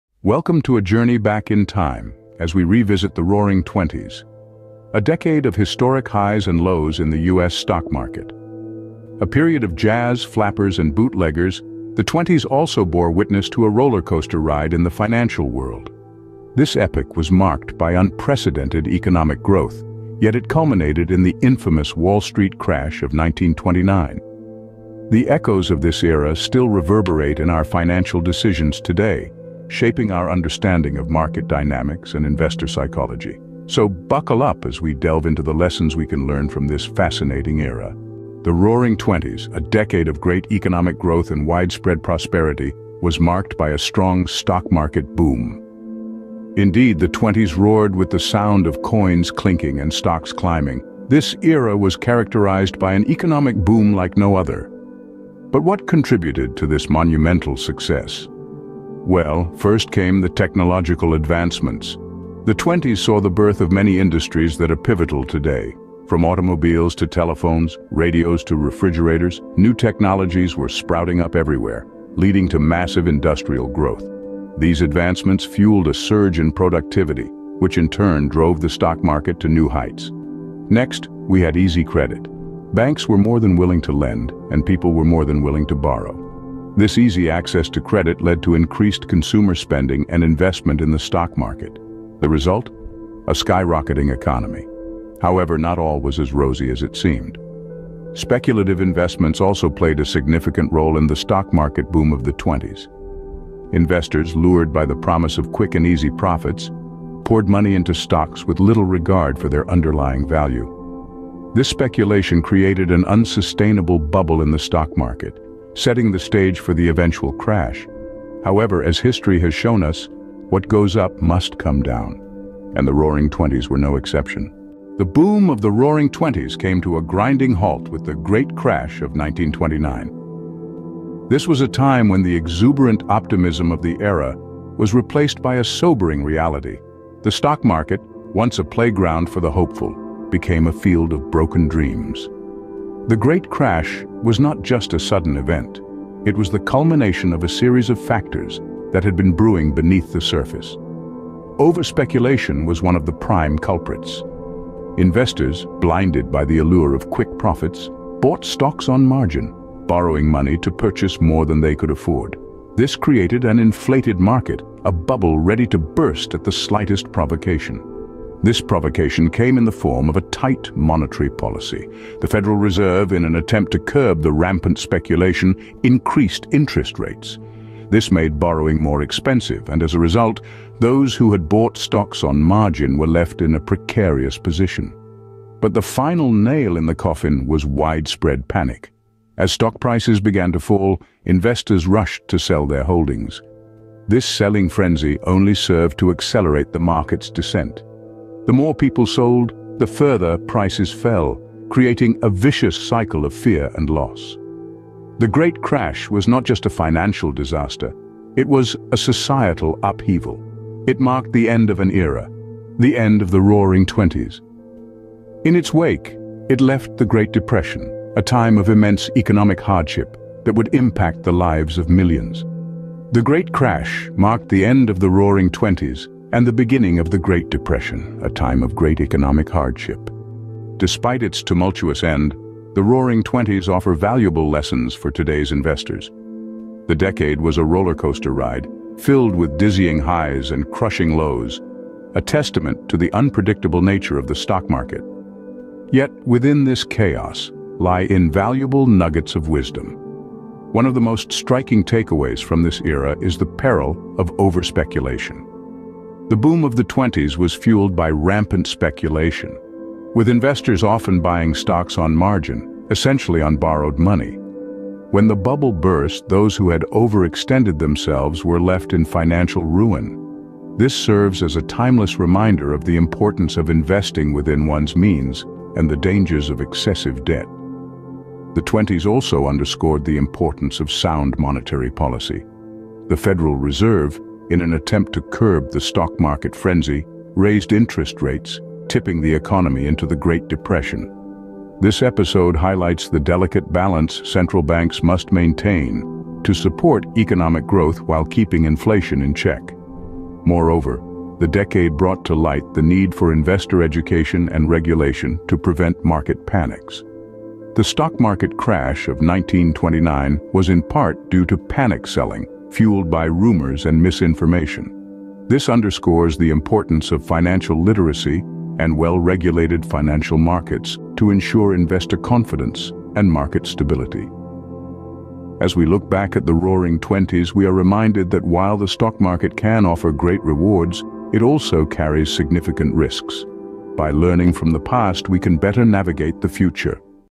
The Roaring Twenties Revisited Lessons from the US Stock Market's Historic Highs and Lows
The Roaring Twenties were a time of unprecedented economic boom and eventual market crash, offering valuable lessons for today's investors. This video delves into the StockMarketHistory of the 1920s, exploring the RoaringTwenties and the 1920sEconomy to uncover MarketLessons and InvestmentLessons. Learn how to avoid the pitfalls of speculation and embrace the importance of fundamentals, RiskManagement, and Diversification. Discover the power of LongTermInvesting and how understanding MarketVolatility can help you navigate today's dynamic markets. By LookingToHistory, you can gain InvestorEducation and FinancialHistory insights that are crucial for USStockMarket success. Join us as we analyze the GreatDepression and the EconomicBoom, and learn how to LearnFromThePast to NavigateTheFuture with confidence and informed InvestmentStrategy.

OUTLINE:

00:00:00 The Introduction
00:00:38 A Time of Boom and Bust
00:02:04 The End of an Era
00:03:50 The Takeaways from the Roaring Twenties